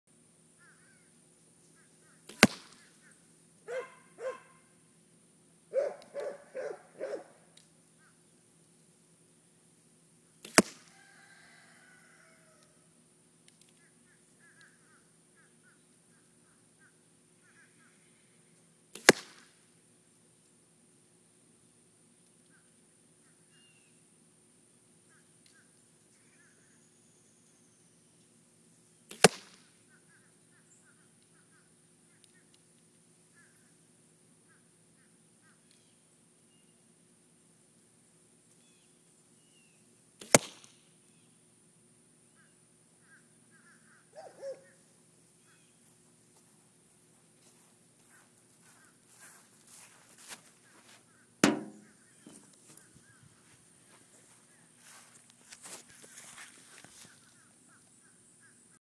How to Hit a Bow Target
Fred tryin to hit the Deerhead logo in the center.